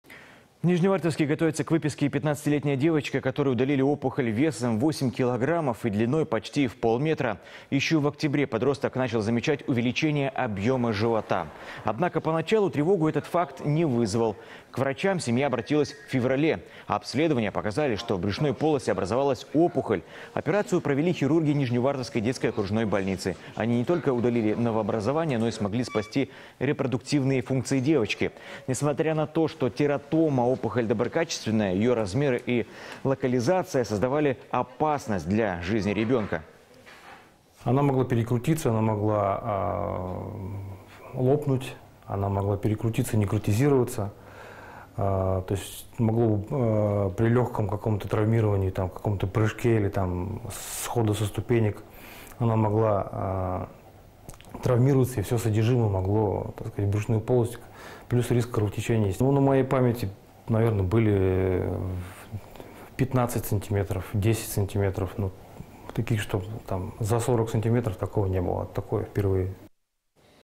В Нижневартовске к выписке готовится пациентка, которой удалили 8-килограммовую опухоль
В Нижневартовске готовится к выписке 15-летняя девочка, которой удалили опухоль весом 8 килограммов и длиной почти в полметра.